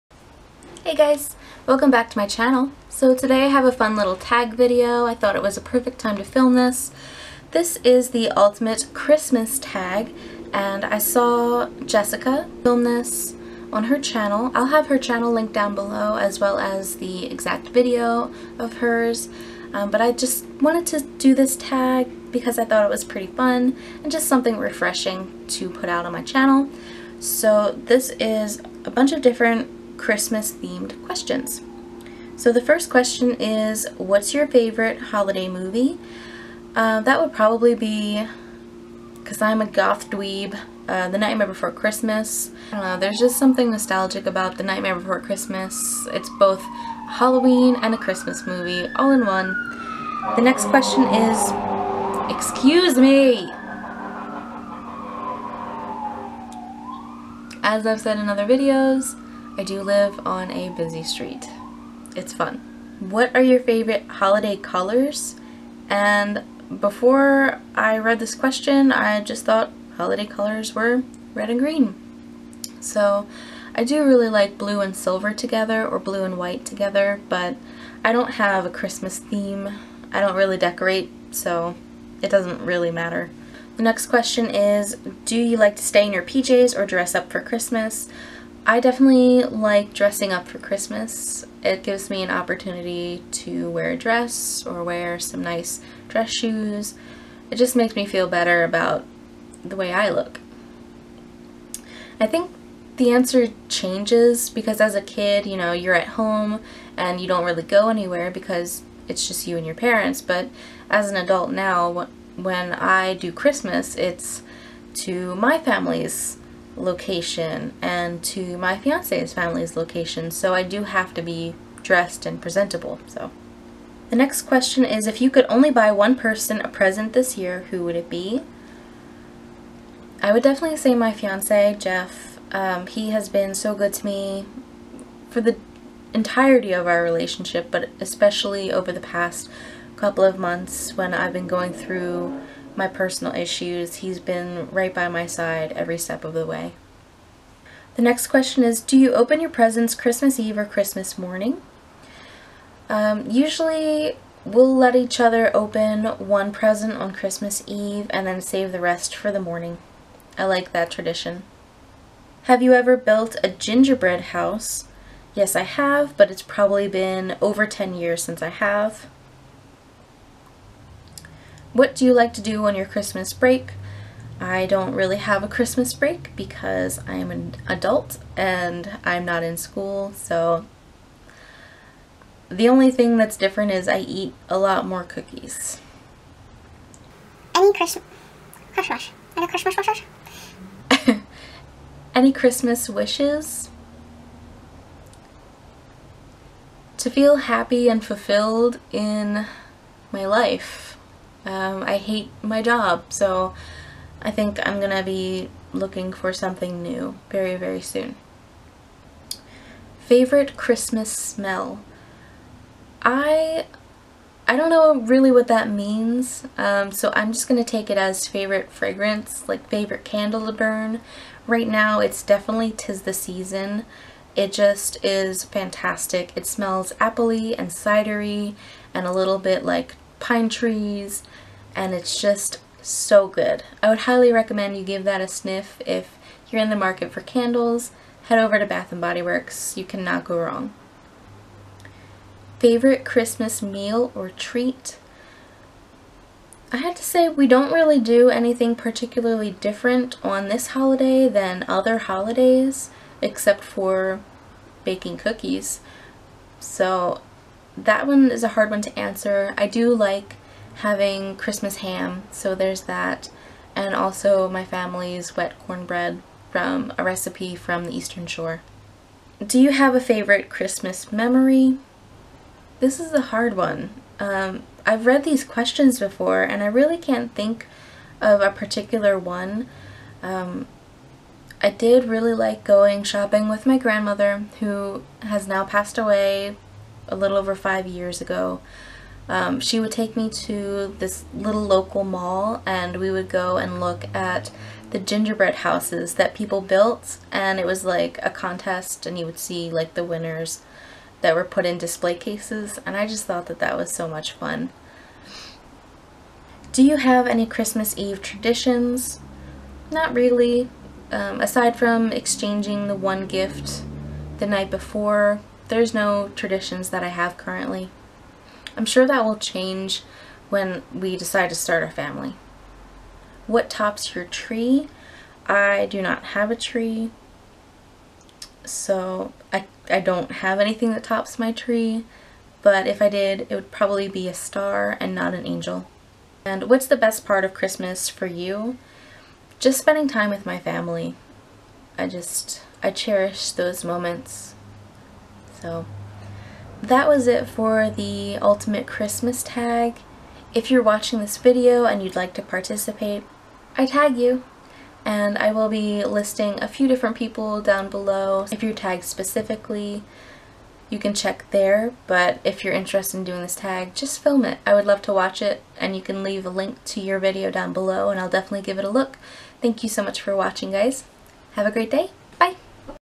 The Ultimate Christmas Tag
Tagging:
Lindsay Sullivan
Jenny / bama_beauty_addict
Fairly Odd Rabbits
Nika Kloobnika
lena7623

Tag questions:
What's Your Favorite Holiday Movie?
What Are Your Favorite Christmas Colors?
Do You Like To Stay in Your PJs Or Dress Up For Christmas?
If You Could Only Buy One Person a Present This Year Who Would It Be?
Do You Open Your Presents Christmas Eve Or Christmas Morning?
Have You Ever Built a Ginger Bread House?
What Do You Like To Do On Your Christmas Break?
Any Christmas Wishes?
Favorite Christmas Smell?
Favorite Christmas Meal Or Treat?
Do you open your presents on Christmas Eve or Christmas morning?
Do you have a favorite Christmas memory?
Do you have any Christmas Eve traditions?
What tops your tree?
What’s the best part about Christmas for you?